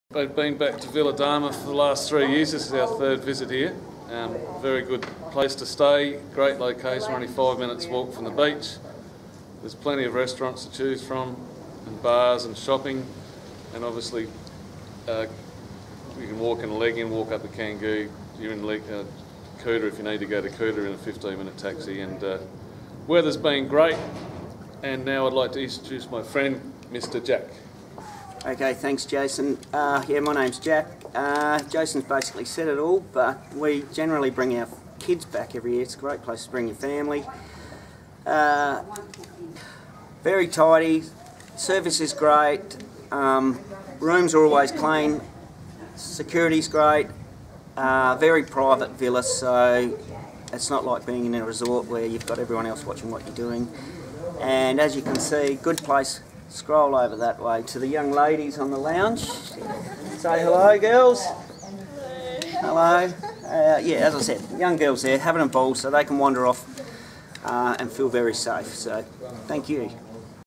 Jack Perkins and friends talks about 'Villa Darma', by Villa Bugis, Seminyak Bali.
A review about Villa Darma - 5 Bedroom Villa in the heart of seminyak.